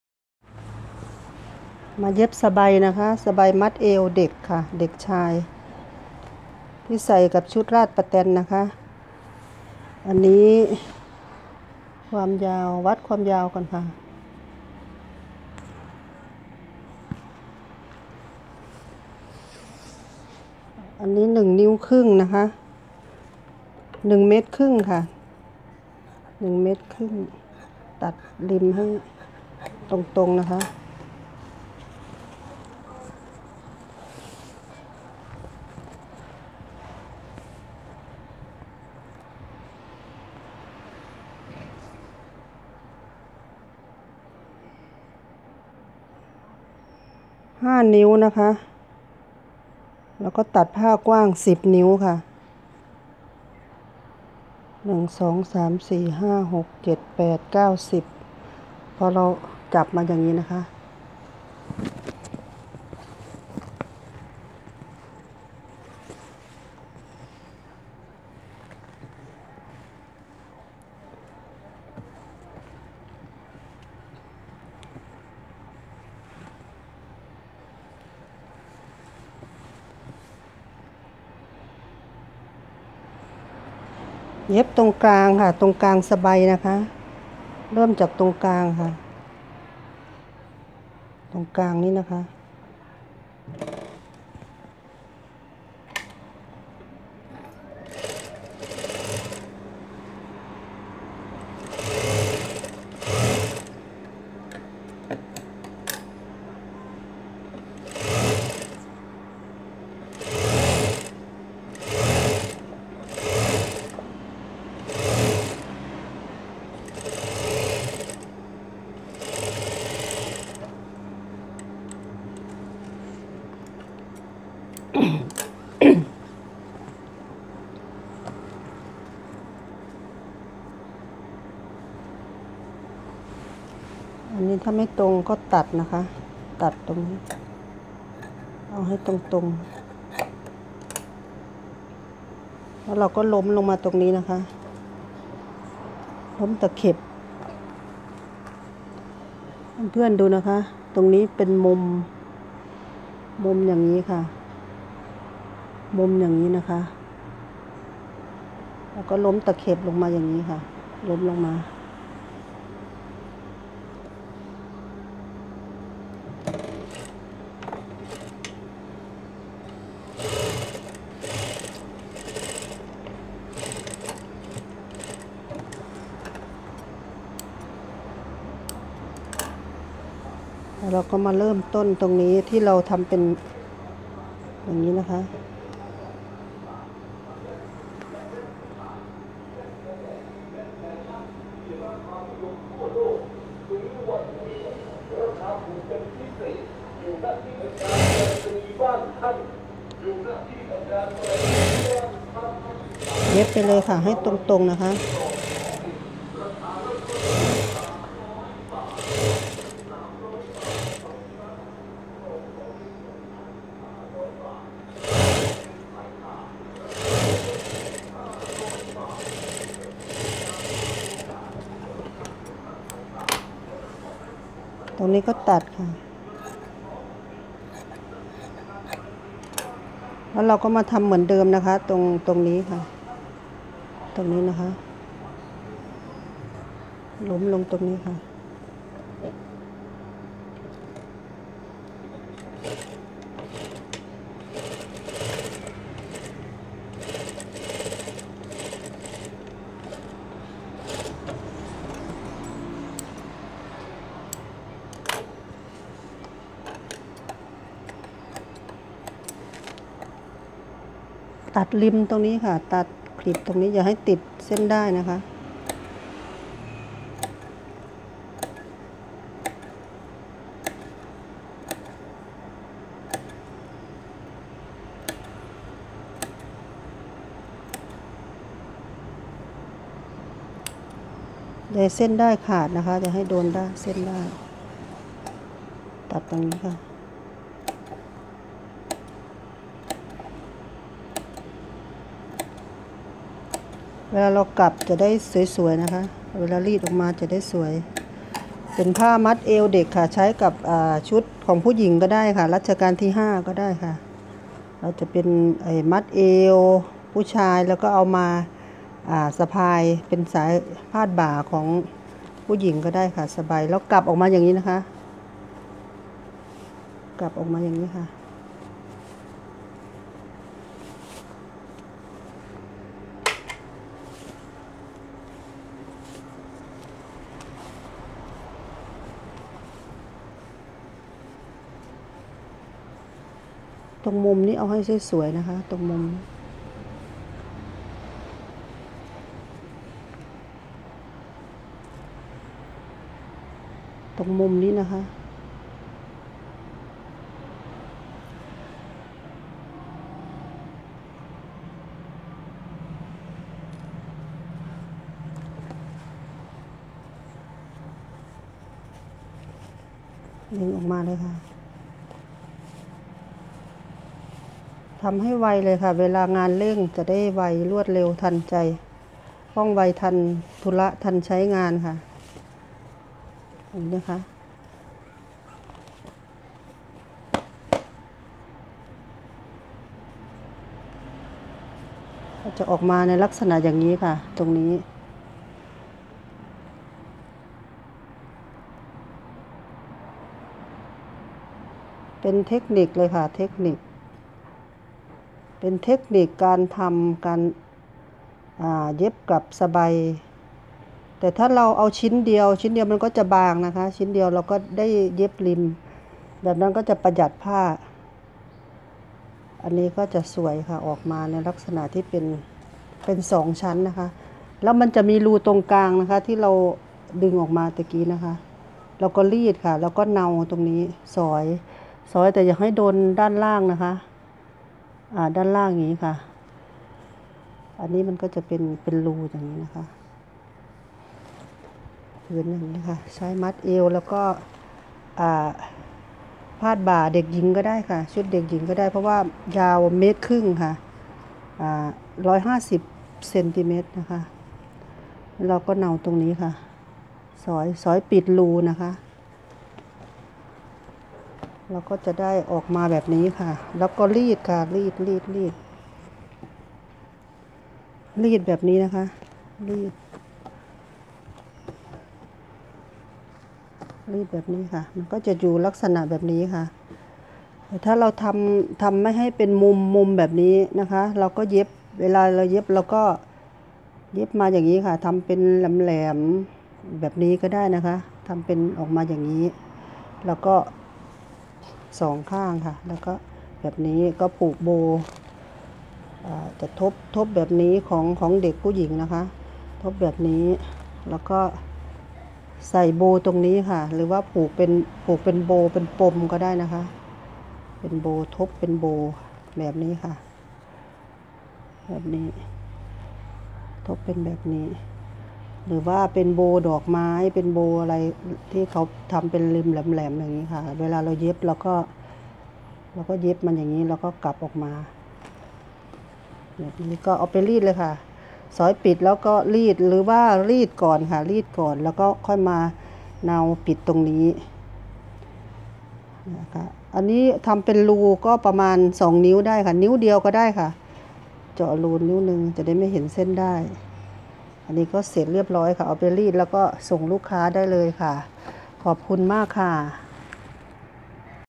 How to วิธีเย็บสไบ ง่ายและรวดเร็ว ชุดไทย
How to วิธีเย็บสไบพาดบ่าคาดเฉียงลงมากับลำตัวกับชุดรัชกาลที่5หรือใช้มัดเอวกับชุดราชประแตน#ประเสริฐศรีชุดไทย